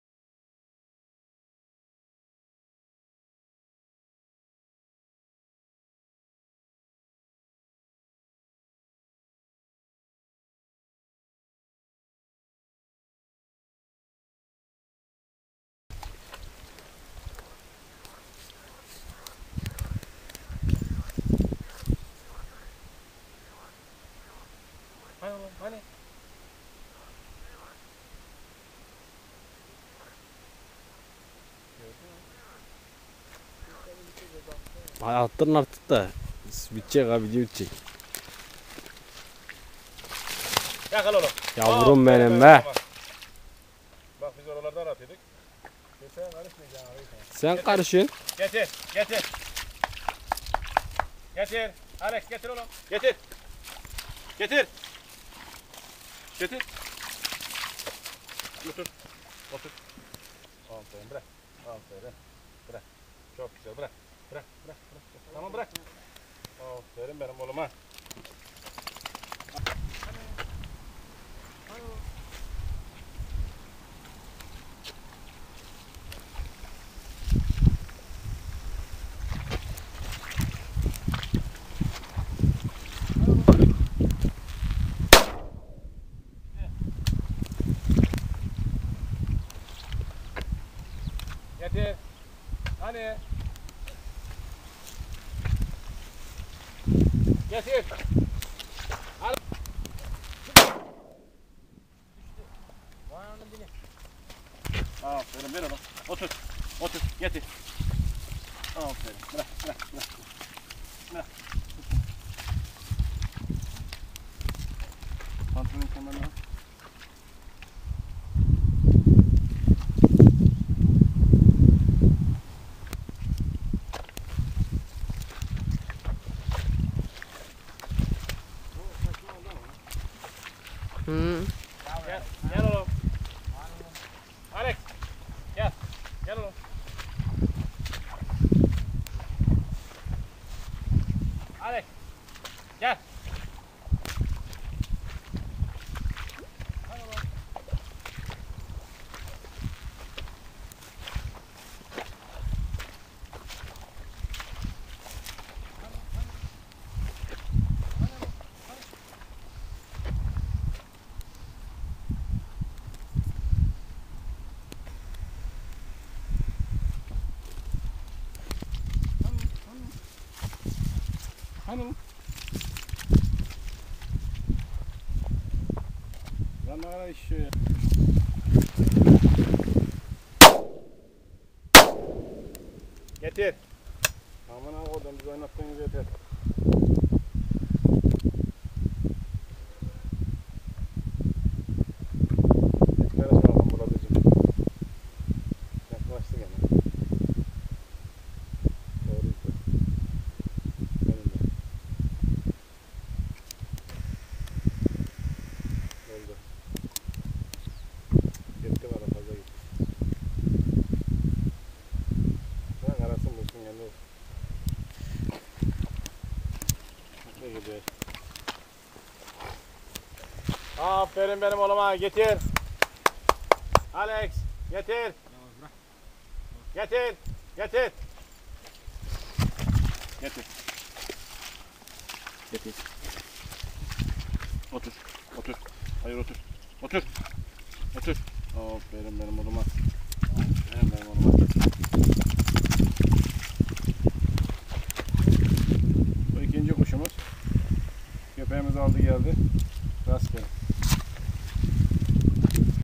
Film 0002
Erkek Macar Vizsla, henüz 11 aylıkken çekilen videosudur. Kardeşleri vardır. Tlf: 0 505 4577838